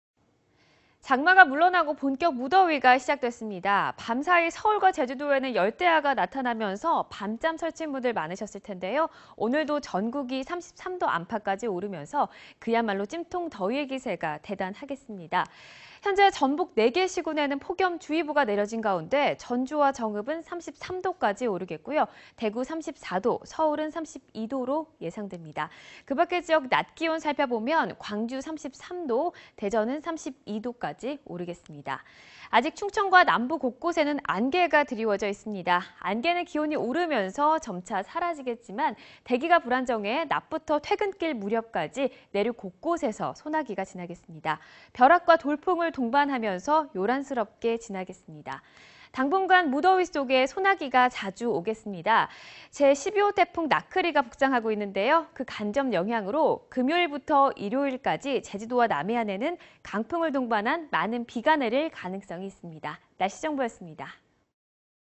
본격 무더위... 서울 32℃, 대구 34℃ (2014-07-30) / YTN 웨더
장마가 물러나고 본격 무더위가 시작됐습니다.밤사이 서울과 제주도에는 열대야가 나타나면서 밤잠 설친 분들 많으셨을 텐데요.오늘도 전국이 33도 안팎까지 오르면서 그야말로 찜통더위의 기세가 대단하겠습니다.현재 전북 4개 시군에는 폭염 주의보가 내려진 가운데 전주와 정읍 33도까지 오르겠고, 대구 34도. 서울 32도...